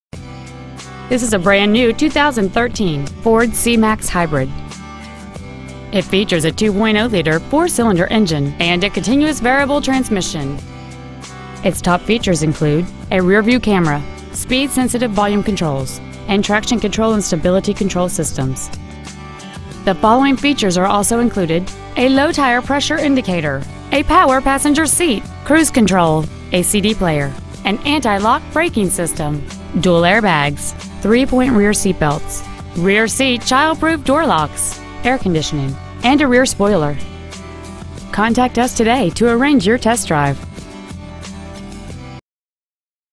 2013 Ford C-Max Hybrid Plainville CT 06062
We are proud to present this 2013 Ford C-Max Hybrid .

 Year : 2013
 Make : Ford
 Model : C-Max Hybrid
 Engine : 2.0L 4 Cyl. Sequential-Port F.I.
 Trans . : Cont. Variable Trans.
 Exterior : Silver
 Miles : 50

 http://
 2013 Ford C-Max Hybrid Plainville CT